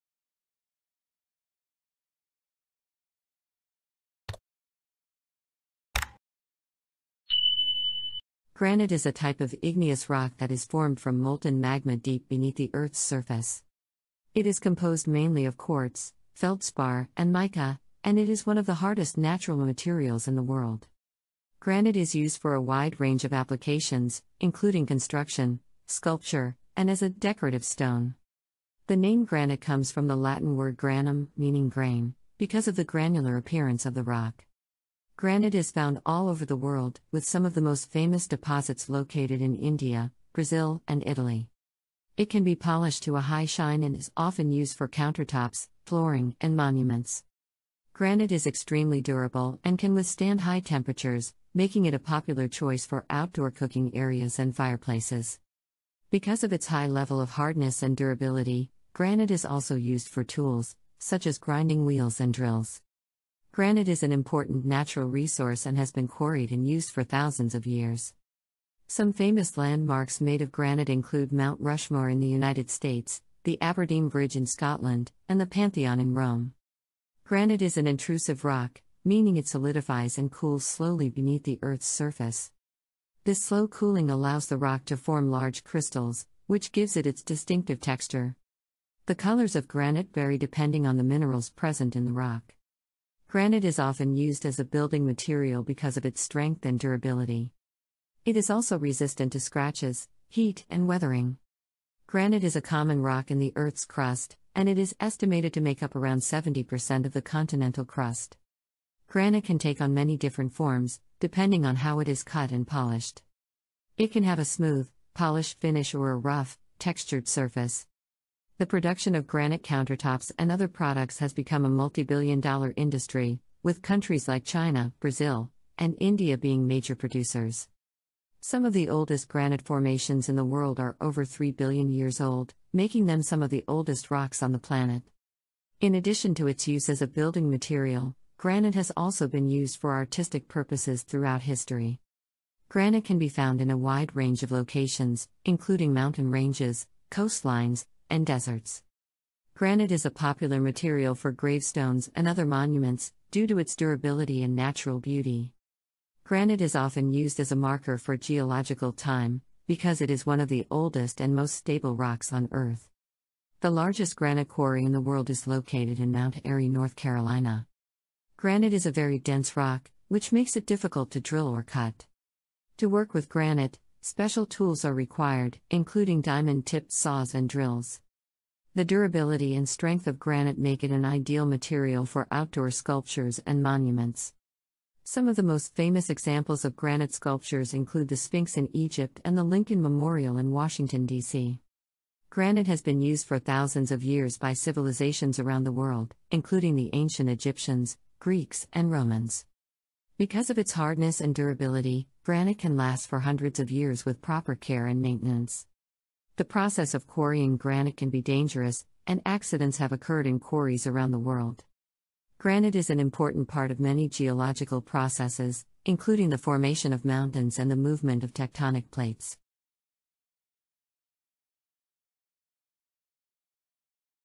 Interesting Facts about Granite
Visit  @BulbOn-zx7ov  for more interesting videos

Granite is a type of igneous rock that is formed from molten magma deep beneath the earth's surface.

It is composed mainly of quartz, feldspar, and mica, and it is one of the hardest natural materials in the world.

Granite is used for a wide range of applications, including construction, sculpture, and as a decorative stone.

The name "granite" comes from the Latin word granum, meaning grain, because of the granular appearance of the rock.

Granite is found all over the world, with some of the most famous deposits located in India, Brazil, and Italy.

It can be polished to a high shine and is often used for countertops, flooring, and monuments.

Granite is extremely durable and can withstand high temperatures, making it a popular choice for outdoor cooking areas and fireplaces.

Because of its high level of hardness and durability, granite is also used for tools, such as grinding wheels and drills.

Granite is an important natural resource and has been quarried and used for thousands of years.

Some famous landmarks made of granite include Mount Rushmore in the United States, the Aberdeen Bridge in Scotland, and the Pantheon in Rome.

Granite is an intrusive rock, meaning it solidifies and cools slowly beneath the earth's surface. This slow cooling allows the rock to form large crystals, which gives it its distinctive texture.

The colors of granite vary depending on the minerals present in the rock. For example, pink and red granite often contain potassium feldspar, while black granite contains more dark minerals like amphiboles and pyroxenes.

Granite is often used as a building material because of its strength and durability. It is also resistant to scratches, heat, and weathering.

Granite is a common rock in the Earth's crust, and it is estimated to make up around 70% of the continental crust.

Granite can take on many different forms, depending on how it is cut and polished. It can have a smooth, polished finish or a rough, textured surface.

The production of granite countertops and other products has become a multi-billion dollar industry, with countries like China, Brazil, and India being major producers.

Some of the oldest granite formations in the world are over 3 billion years old, making them some of the oldest rocks on the planet.

In addition to its use as a building material, granite has also been used for artistic purposes throughout history. The ancient Egyptians, for example, used granite to create statues and other works of art.

Granite can be found in a wide range of locations, including mountain ranges, coastlines, and deserts.

Granite is a popular material for gravestones and other monuments, due to its durability and natural beauty.

Granite is often used as a marker for geological time, because it is one of the oldest and most stable rocks on Earth.

The largest granite quarry in the world is located in Mount Airy, North Carolina. It is over 600 feet deep and covers an area of 60 acres.

In addition to its natural beauty, granite is also valued for its radiation-resistant properties, which makes it ideal for use in medical equipment and nuclear facilities.

Granite is a very dense rock, which makes it difficult to drill or cut. To work with granite, special tools are required, including diamond-tipped saws and drills.

The durability and strength of granite make it an ideal material for outdoor sculptures and monuments. Some of the most famous examples of granite sculptures include the Sphinx in Egypt and the Lincoln Memorial in Washington, D.C.

Granite has been used for thousands of years by civilizations around the world, including the ancient Egyptians, Greeks, and Romans.

Because of its hardness and durability, granite can last for hundreds of years with proper care and maintenance.

The process of quarrying granite can be dangerous, and accidents have occurred in quarries around the world. Safety measures are now in place to minimize the risk of accidents.

Granite is an important part of many geological processes, including the formation of mountains and the movement of tectonic plates.